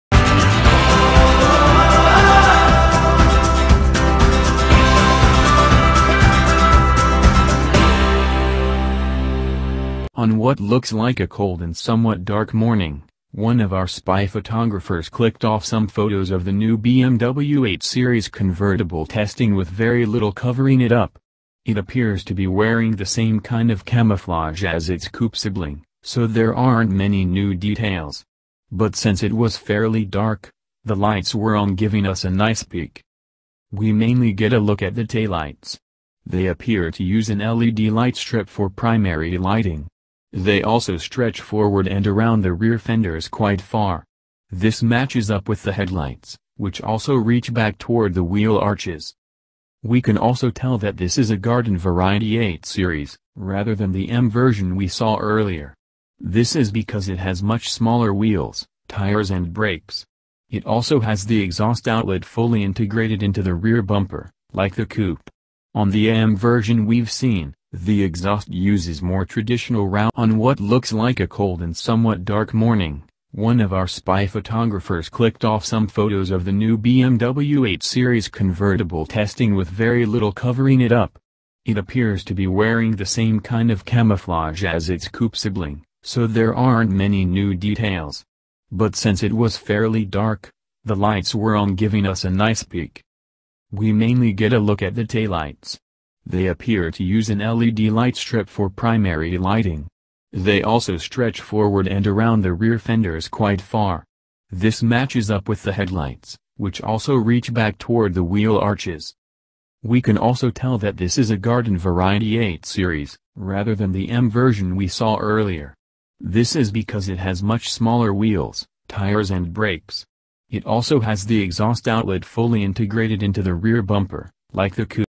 POWERCARS # 2020 BMW 8 Series Convertible photos show flagship drop top is nearly ready
On what looks like a cold and somewhat dark morning, one of our spy photographers clicked off some photos of the new BMW 8 Series Convertible testing with very little covering it up. It appears to be wearing the same kind of camouflage as its coupe sibling, so there aren't many new details. But since it was fairly dark, the lights were on giving us a nice peek.